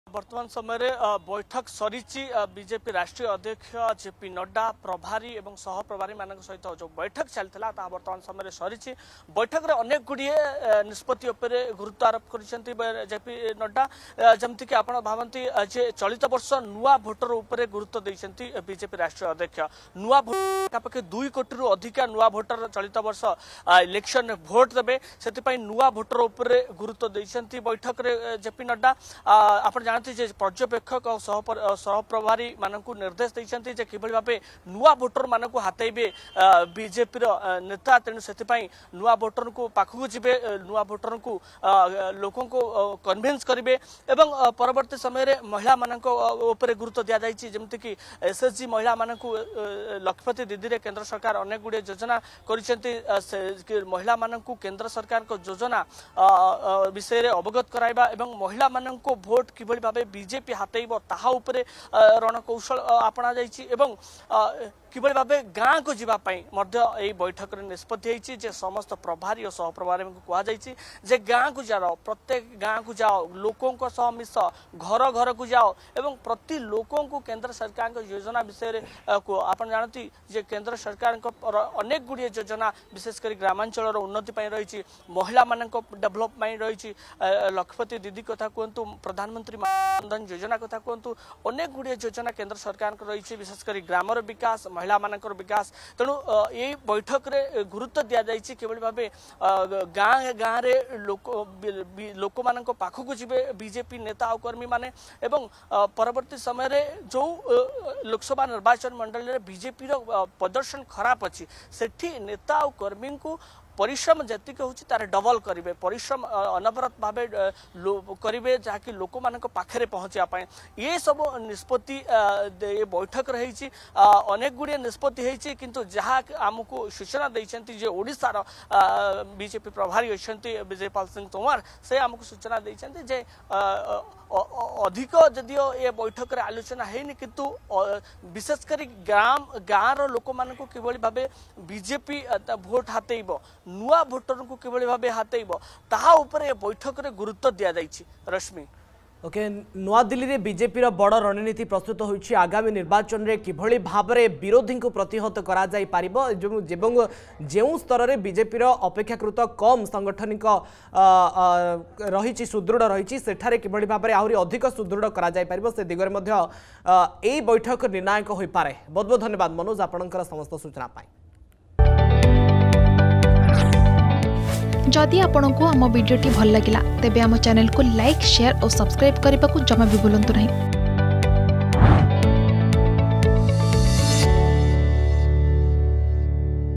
ନୂଆଦିଲ୍ଲୀରେ ବିଜେପିର ବଡ଼ ରଣନୀତି || Election 2024 Preparation || BJP
ନୂଆଦିଲ୍ଲୀରେ ବିଜେପିର ବଡ଼ ରଣନୀତି। ବିଜେପି ମୁଖ୍ୟ କାର୍ଯ୍ୟାଳୟରେ ନିର୍ବାଚନ ପ୍ରଭାରୀଙ୍କ ସହ ଆଲୋଚନା ଶେଷ। ବିଜେପି ଆଗାମୀ ସାଧାରଣ ନିର୍ବାଚନ ପ୍ରସ୍ତୁତି ପାଇଁ ଏହି ବୈଠକ ବସିଥିଲା ।